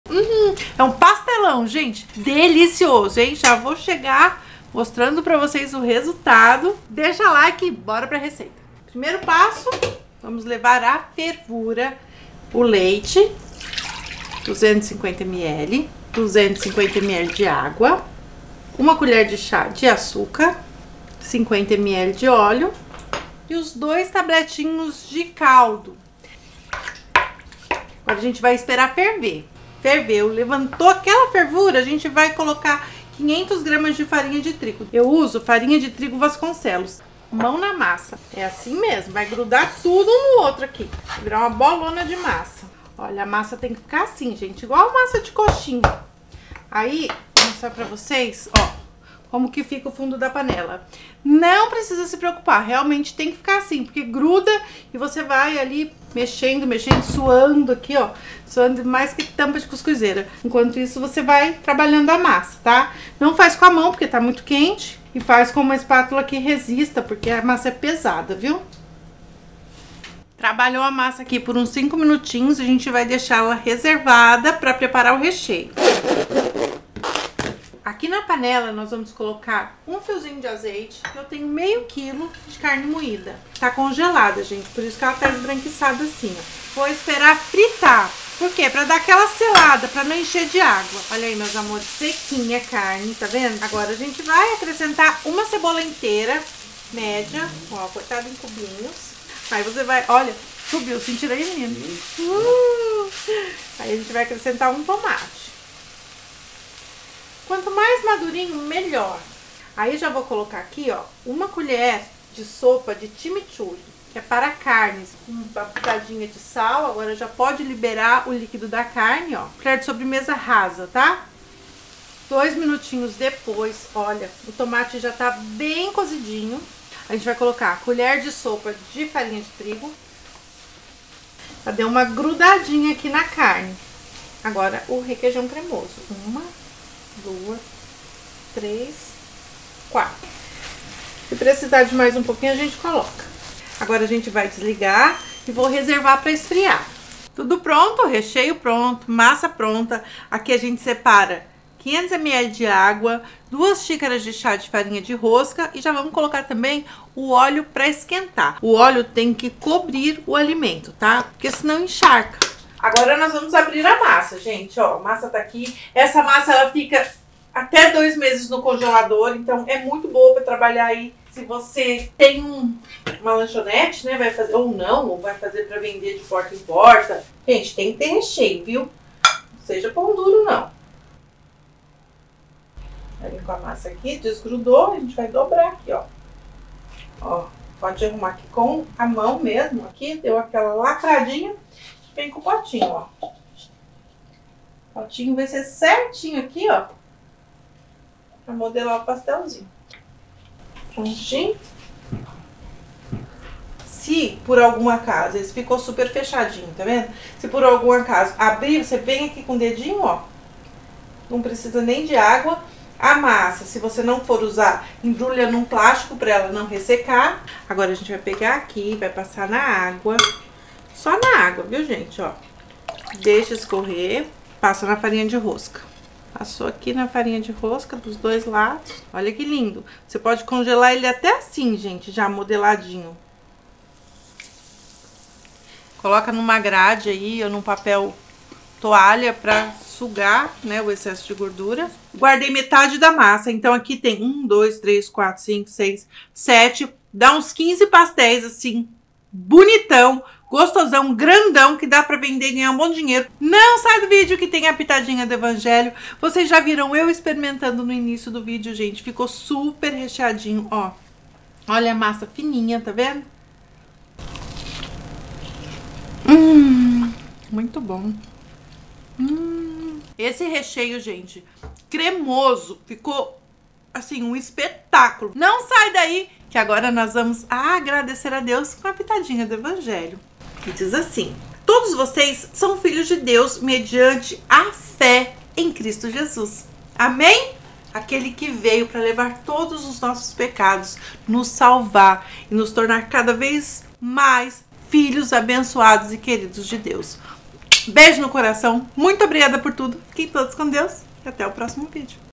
PASTELÃO CREMOSO: UMA RECEITA IRRESISTÍVEL PARA SABOREAR EM CASA!
Pastelão Cremoso! Descubra o segredo por trás desse sabor irresistível que você pode saborear em casa. 😋🥟

Recheio:
Fio de azeite
Meio quilo de carne moída (congelada)
1 cebola inteira média (cortada em cubinhos)
1 tomate (maduro)
1 colher de sopa de chimichurri (tempero para carnes)
1 colher de sopa de farinha de trigo
Requeijão cremoso

Massa:
250 ml de leite
250 ml de água
1 colher de chá de açúcar
50 ml de óleo
2 tabletes de caldo
1 colher de sobremesa rasa de sal
500 gramas de farinha de trigo